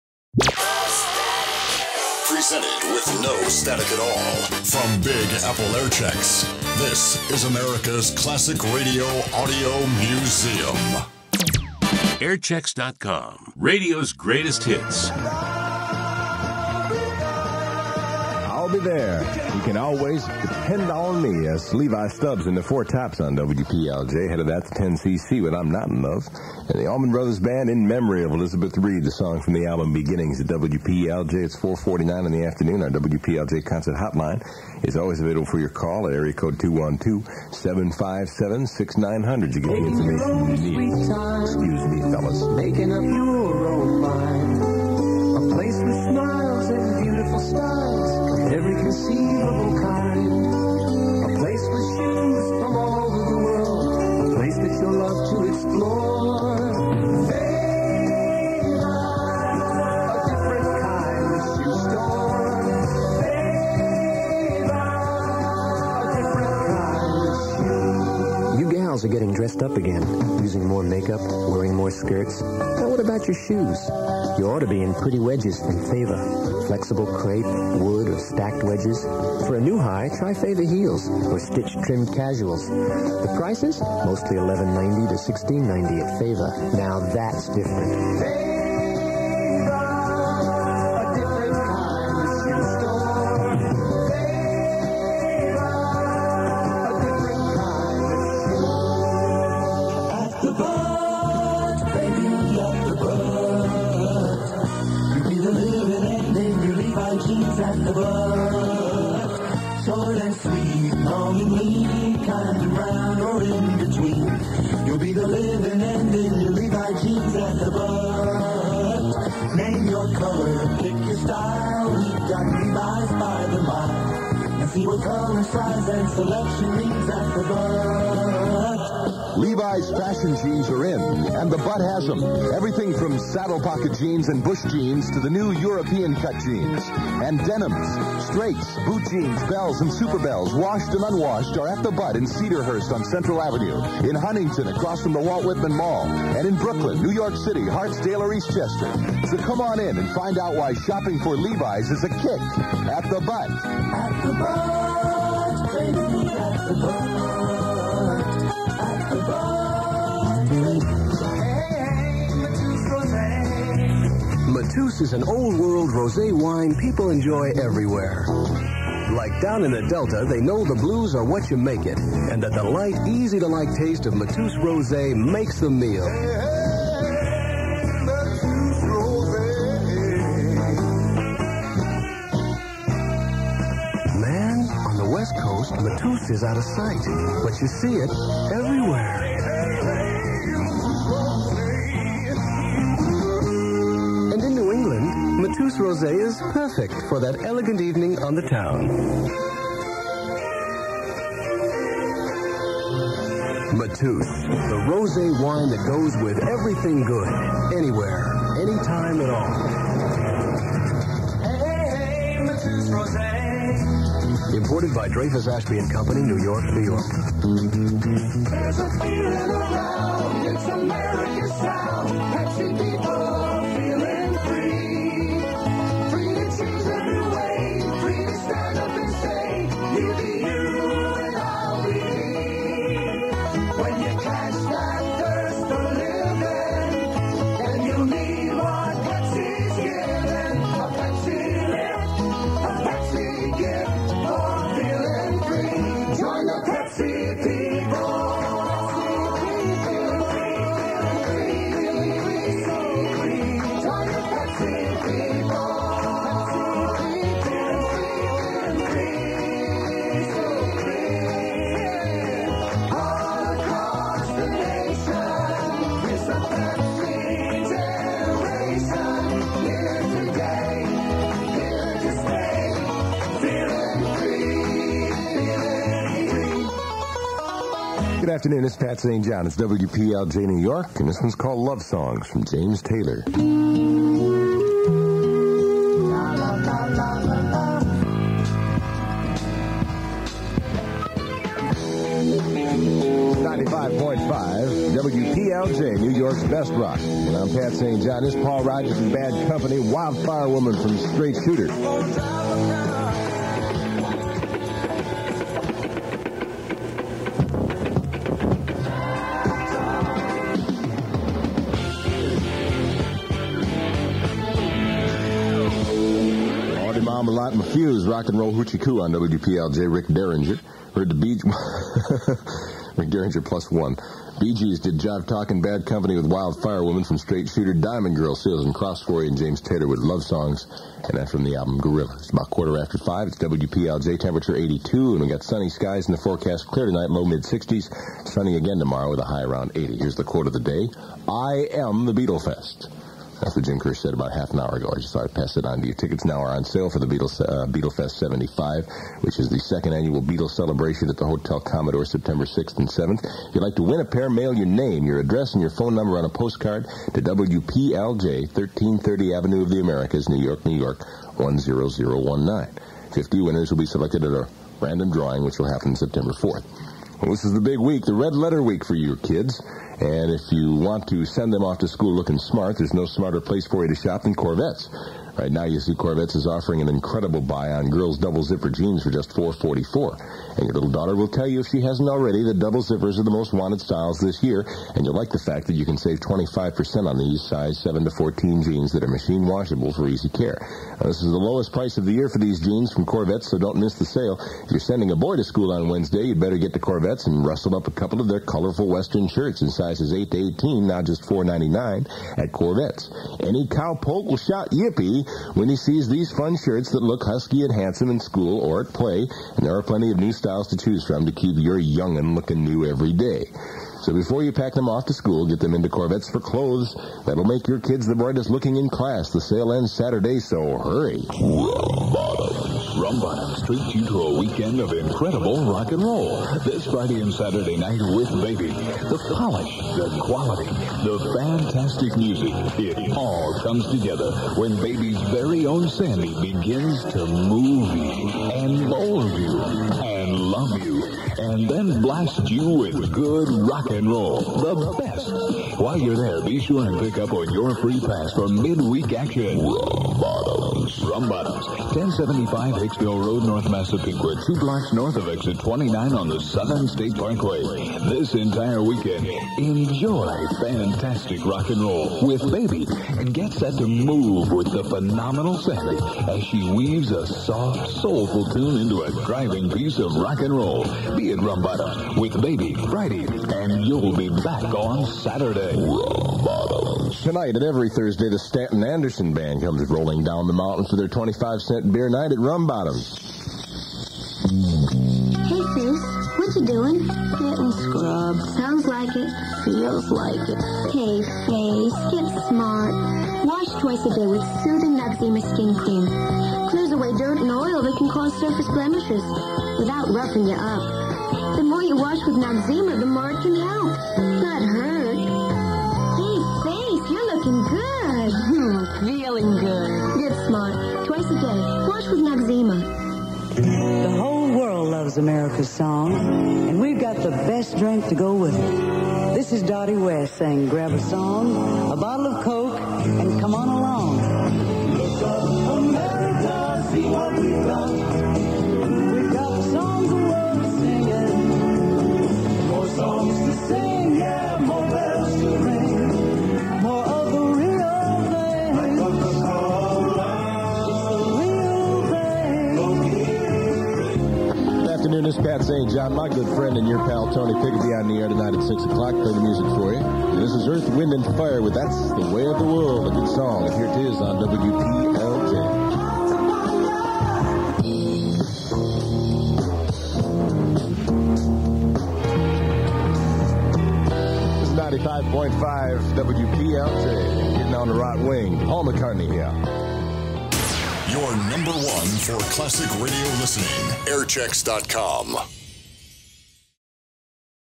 Pat St. John, 95.5 WPLJ New York - 08.28.1975
This telescoped aircheck really doesn't do WPLJ justice.  It's somewhat difficult to convey the sound of the station with all the music taken out, except for a few segues.

Most people remember WPLJ as one of three album rock (AOR) formatted stations in the New York City area.  The audio image most think of when one mentions AOR, is usually the hard rock arena bands of the 1970s, such as The Who, Led Zeppelin, Heart and others.  But here in this just over 9 minute aircheck, you'll notice that WPLJ sounds a lot more like a soft rock station than a traditional AOR - or even a Classic Rock station today.  Earth Wind & Fire, James Taylor, The Bee Gees... that's what was on the WPLJ playlist this particular August afternoon.

Matt at BAA suggested that perhaps WPLJ was dayparted, meaning perhaps certain hours of the day the station played harder edged music than others, and that's certainly a possibility.  It's more likely, however, that WPLJ evolved into the harder Rock station that most remember over the course of time.   And in 1983, the station would ditch Album Rock for CHR and then AC in the 90s - and never return to the original format.

for more classic radio airchecks!